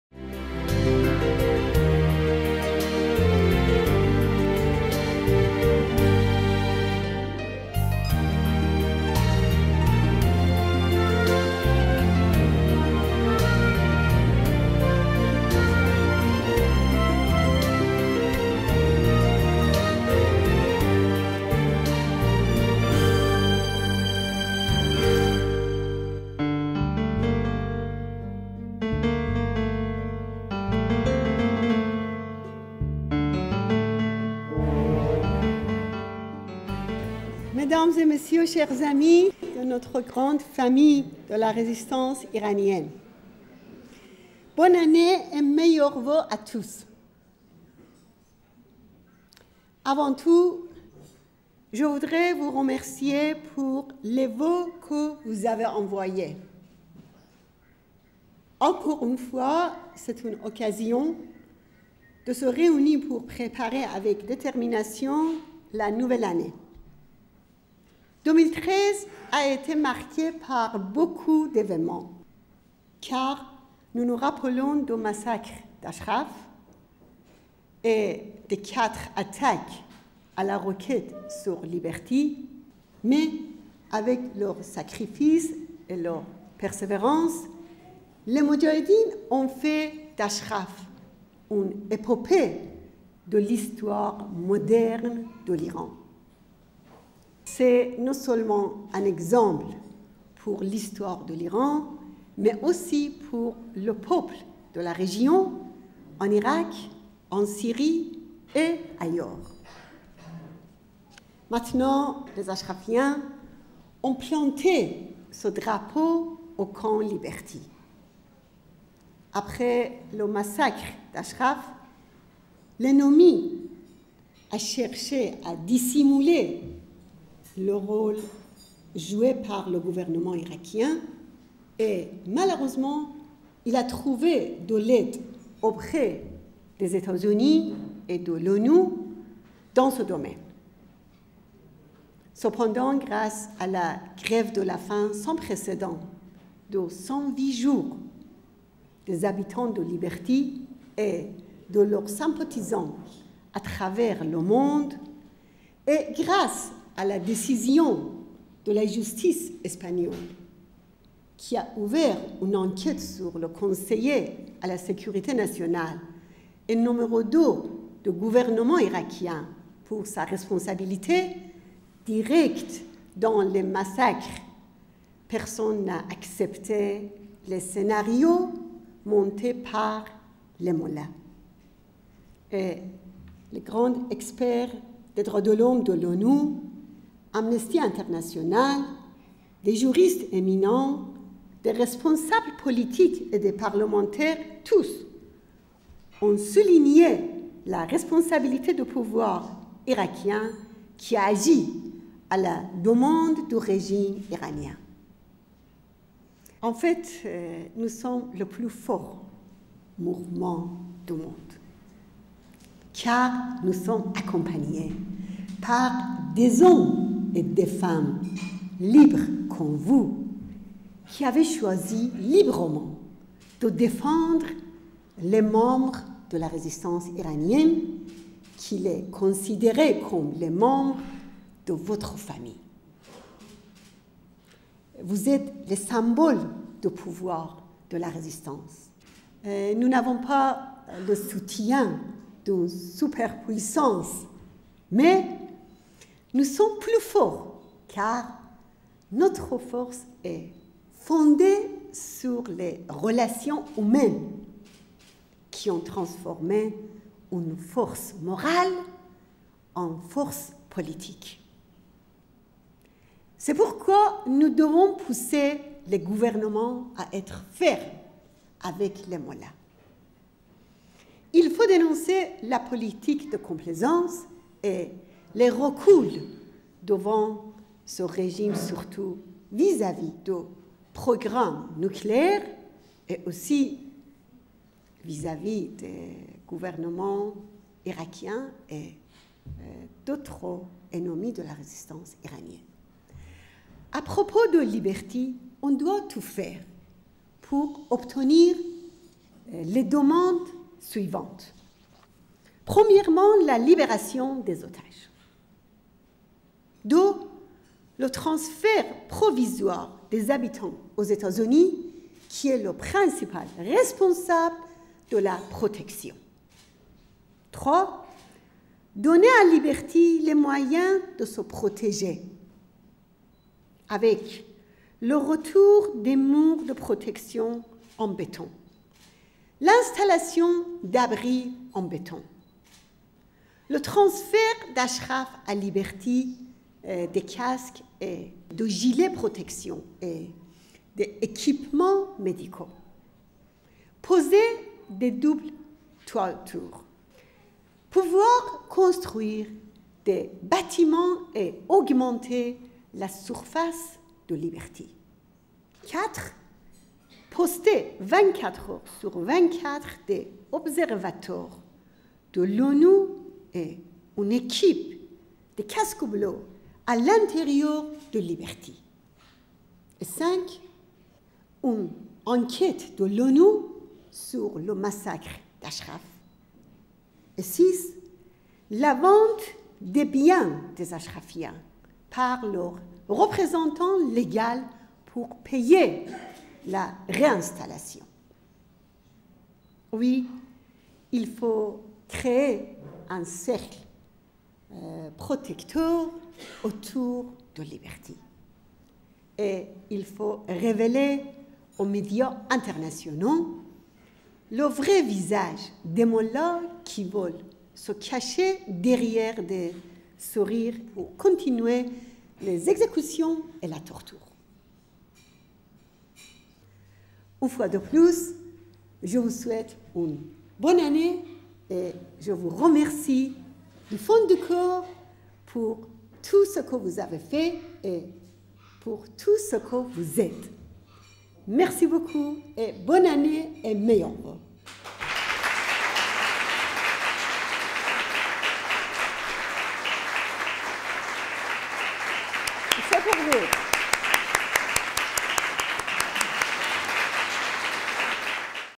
Vœux de Maryam Radjavi aux Auversois et Val d'Oisiens 12 janvier 2014
Maryam Radjavi a présenté dans une cérémonie des voeux ses souhaits pour une nouvelle année aux français et aux habitants de Val d'Oise.
Elle a dit: 2013 a été marquée par beaucoup d'événements. D'abord, viennent des images douloureuses dans nos mémoires, car nous nous rappelons du massacre d'Achraf et des quatre attaques à la roquette sur Liberty. Nous avons perdu beaucoup de frères et sœurs et chacun avait dans notre cœur une place unique. Des femmes et des hommes qui ont forgé la grande épopée de la résistance et des valeurs humaines. Ils ont consacré toute leur vie à la liberté de leur peuple et de leur pays enchainé. Il y a quelques jours, nous avons rendu hommage à leur mémoire et nous avons promis de continuer leur chemin.

Mais au-delà de cette réalité douloureuse, il y a d'autres points que je voudrais partager avec vous. Avant tout, il faut savoir que ces attaques visent à faire capituler les membres de la Résistance iranienne. Mais avec leur sacrifice et leur persévérance, les Moudjahidine du peuple ont fait d'Achraf une épopée de l'histoire moderne de l'Iran. C'est non seulement un exemple pour l'histoire de l'Iran mais aussi pour les peuples de la région, en Irak, en Syrie et ailleurs.